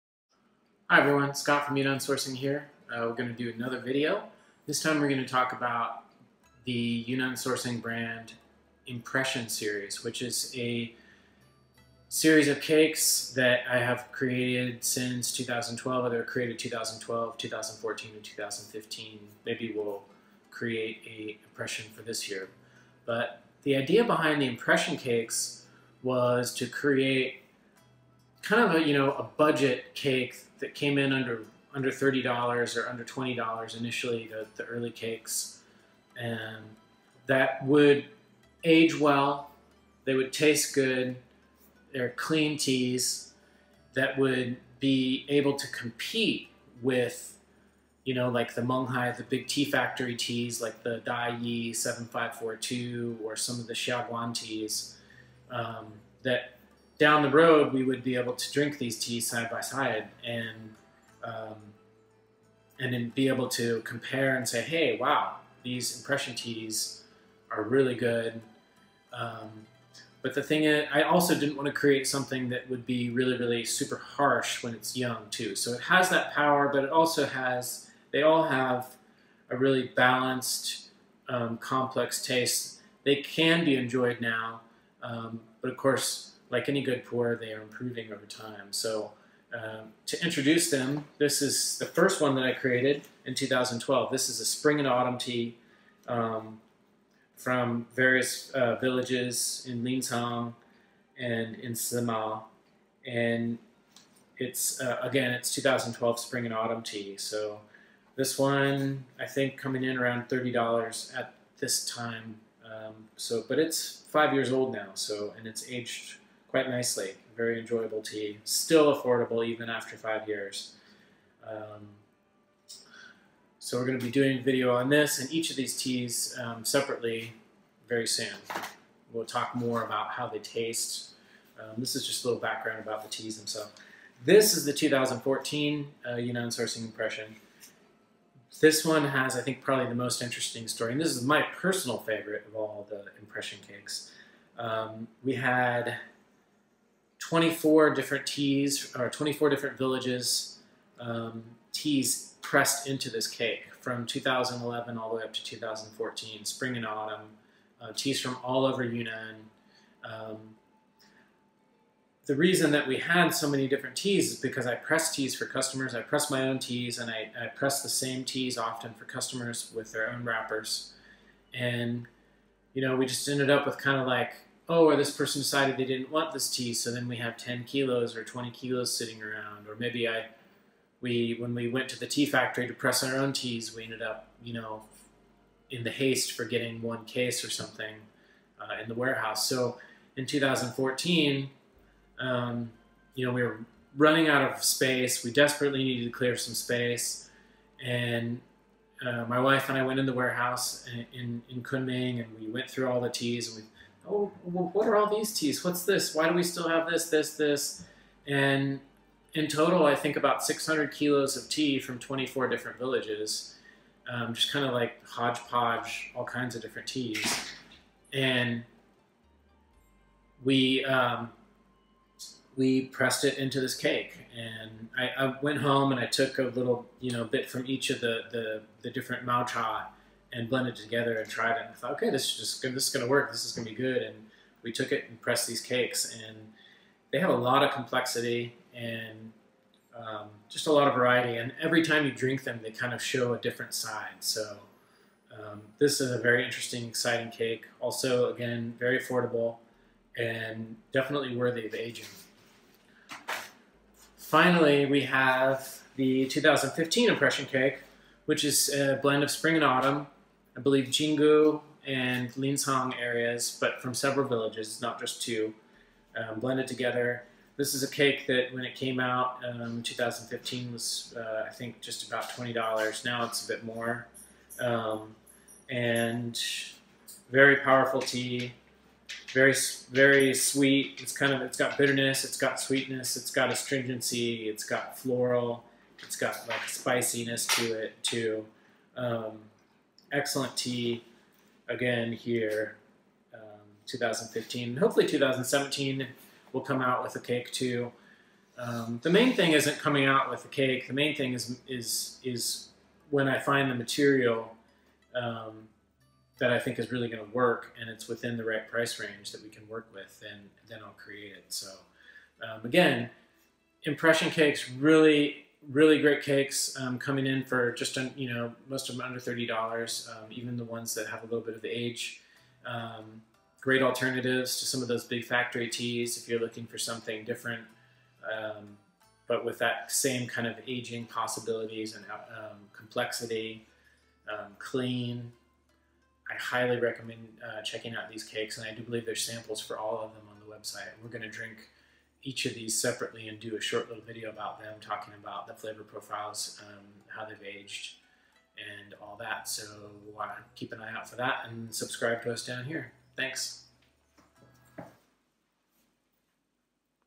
Introducing the Yunnan Sourcing Impression Raw Pu-erh Tea Cake Series
2012 Yunnan Sourcing "Impression" Raw Pu-erh Tea Cake

Our new blend is composed of spring and autumn 2012 plantation and wild arbor mao cha from Lincang, Wu Liang and Simao.  This is our first attempt at creating blend suitable for long-term aging that is priced affordably.  A variety of teas with different characteristics were tirelessly blended together to create a nice balanced tea that has both bitterness and astringency, as well as a nice taste and good cha qi.  The brewed tea is a vibrant floral affair, with very slight smoky notes and strong caramel sugarcane aroma and taste.

357 grams per cake (7 cakes per bamboo leaf tong)

** This production is suitable for re-sellers of tea who prefer to sell tea without Yunnan Sourcing logos and markings.  The wrapper and nei fei make no reference to Yunnan Sourcing whatsoever! **

2014 Yunnan Sourcing Impression Raw Pu-erh Tea Cake

This year's Impression cake shares some similarities with it's 2012 counterpart.  It's a blend of different teas, but this year we used mao cha from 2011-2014 (both Spring and Autumn) harvests.  The mao cha we used is a blend of 24 distinct areas, including Yi Wu, Menghai, Lancang, Jing Gu, Wu Liang, Ai Lao, Mengku, Yong De, Bang Dong, Mangshi (Dehong) and Pu'Er.  Being a blend of so many areas I really feel that this tea has just about everything to offer the drinker.  It is bitter and astringent, but very balanced in that.  It lasts very long in the mouth and throat and is very infusable.  It's exciting to notice the different teas taking their turn at center stage (much a like live Jazz solo improvisation) during the long brewing session.

In 2012 I created the first impression blend to be an alternative to a Xiaguan or Da Yi 7542, but now (especially this year's blend) it has far surpassed those mass market teas in both quality and value!  I also feel strongly that this tea being very strong in aroma, mouthfeel, bitter/astringent, infusability and Cha Qi, makes it a good choice for long-term aging!

Stone-pressed in the traditional manner.

357 grams per cake (7 cakes per bamboo leaf tong)

2015 Yunnan Sourcing Impression Raw Pu-erh Tea Cake

This year's Impression is unique for this year and consists of tea leaves from Spring and Autumn, originating from both Lincang and Jing Gu tea gardens. The tea has been grown naturally and processed in the traditional method. We blended various teas together to achieve a powerful blend that has strong mouth feeling, cha qi and a balanced sweet, bitter and astringent profile.

In 2012 I created the first impression blend to be an alternative to a Xiaguan or Da Yi 7542, but it has far surpassed those mass market teas in both quality and value!  I also feel strongly that this tea being very strong in aroma, mouthfeel, bitter/astringent, infusability and Cha Qi, makes it a good choice for long-term aging!

357 grams per cake (7 cakes per bamboo leaf tong)